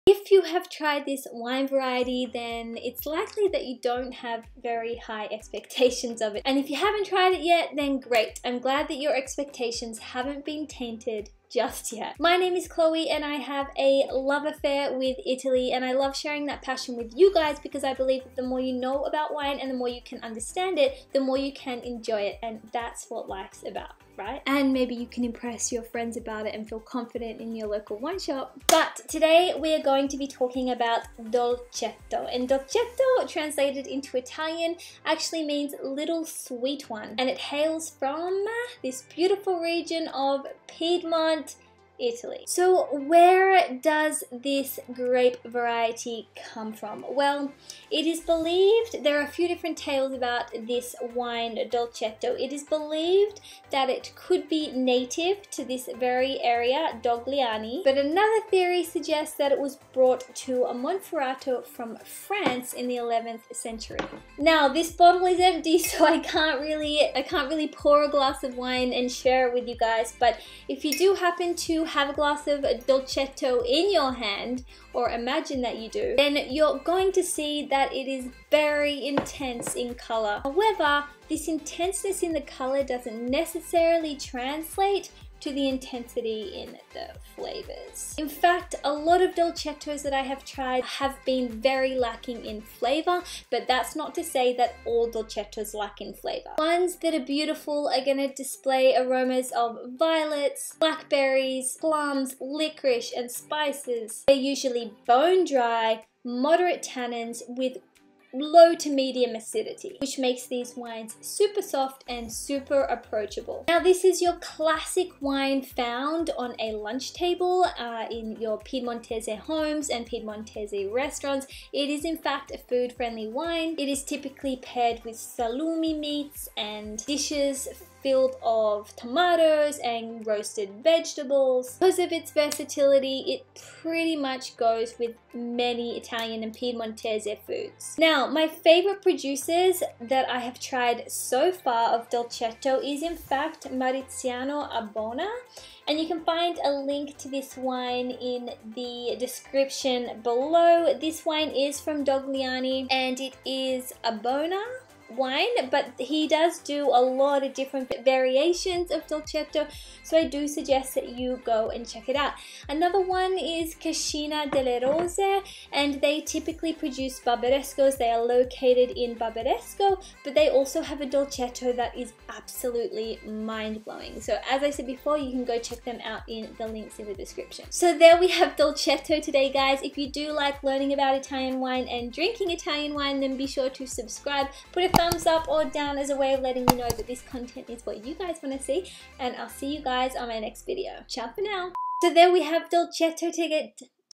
WHAT IS DOLCETTO | 3 Minute Wine Guide To This Red Italian Grape Variety From Piedmont Italy.
If you HAVE tried Dolcetto then it is likely you don't have high expectations of it - and if you HAVEN'T tried it yet - great. I'm glad your expectations haven’t been tainted just yet. Check this 3-minute wine guide to learn about this (supposedly) native grape variety of Piedmont, Italy. P.S To all the Italian's watching, I'm sorry I butchered the name of Dogliani... my ears are hurting too.